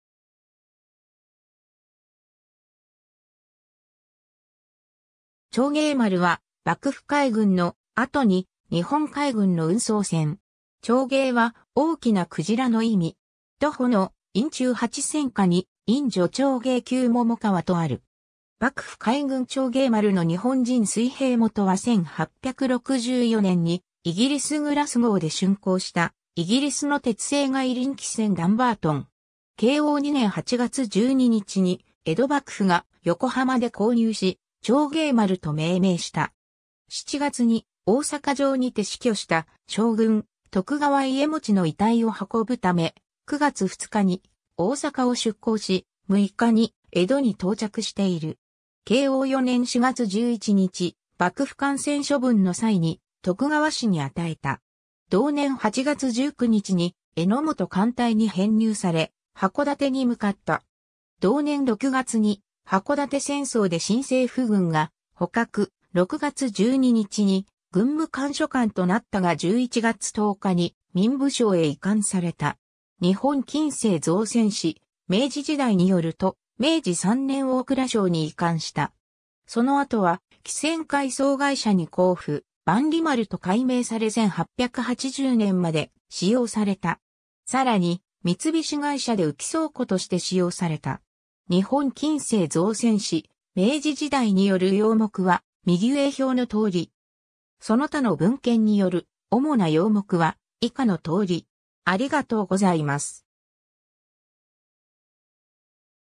長鯨丸
長鯨丸(ちょうげいまる/ちゃうげいまる)は、幕府海軍の、後に日本海軍(軍務官直轄)の運送船(運輸船)。
 長鯨は「大きな鯨」の意味。
 杜甫の『飲中八仙歌』に「飲如長鯨吸百川」(飲ムコト長鯨ノ 百川（ひゃくせん）ヲ吸フガ如シ)とある。
 幕府海軍長鯨丸の日本人水兵(1867年、ジュール・ブリュネ作) 元は1864年(元治元年)にイギリス・グラスゴーで竣工したイギリスの鉄製外輪汽船DANBERTON(ダンバートン、ドムバルトン)。
 慶応2年(1866年)8月12日に江戸幕府が横浜で購入し、 長鯨丸と命名した。
 7月に大阪城にて死去した将軍徳川家茂の遺体を運ぶため、9月2日に大阪を出航し、6日に江戸に到着している。
 慶応4年(明治元年)4月11日、幕府艦船処分の際に徳川氏(静岡藩)に与えた。
 同年8月19日に榎本艦隊に編入され、函館に向かった。
 同年6月に箱館戦争で新政府軍が捕獲、6月12日に軍務官所管となったが11月10日に民部省へ移管された。
 『日本近世造船史 明治時代』によると、明治3年大蔵省に移管した。
 その後は汽船回漕会社に交付、万里丸(萬里丸)と改名され1880年(明治13年)まで使用された。
 更に三菱会社で浮き倉庫(ハルク)として使用された。
 『日本近世造船史 明治時代』による要目は右上表の通り。
 その他の文献による主な要目は以下の通り。